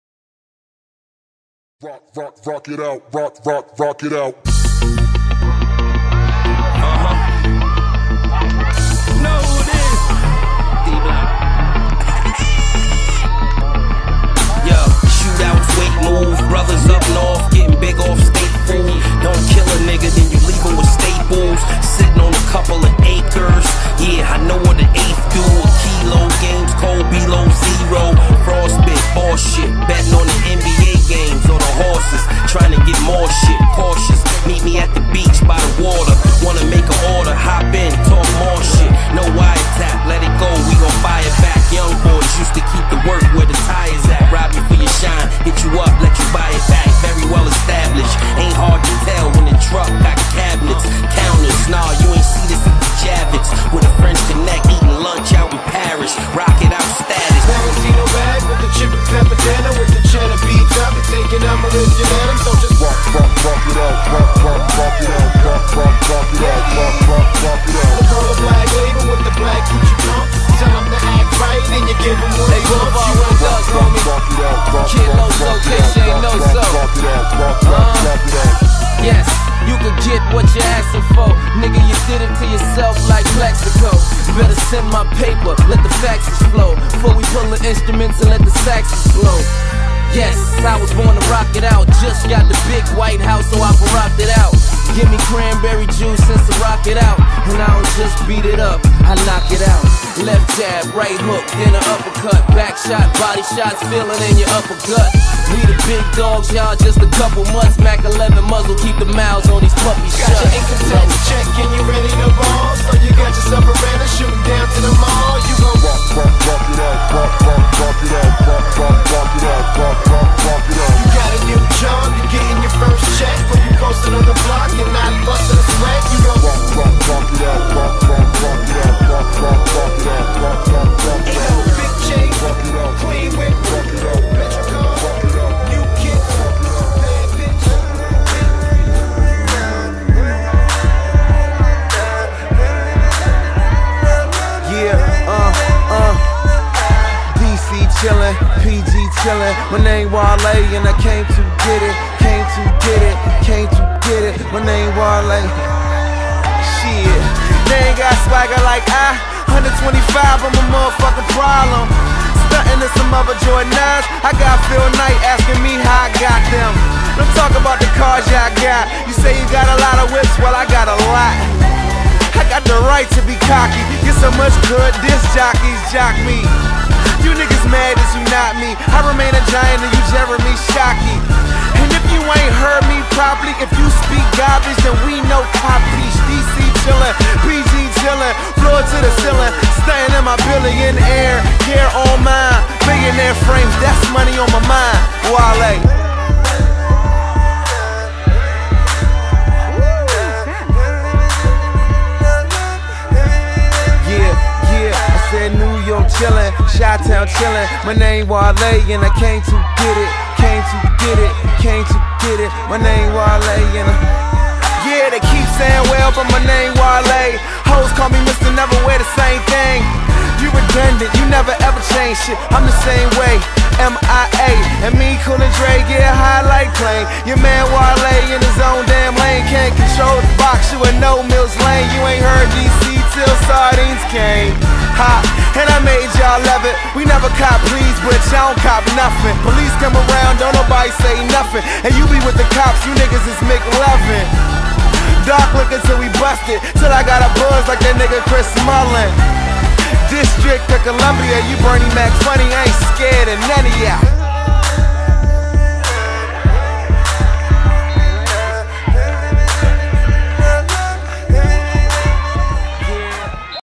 Jamahl Knowles Football Highlights 2008
Jamahl knowles 2008 Football hightlights with the BCFC's Big Kahuna Rams.CJFL Football.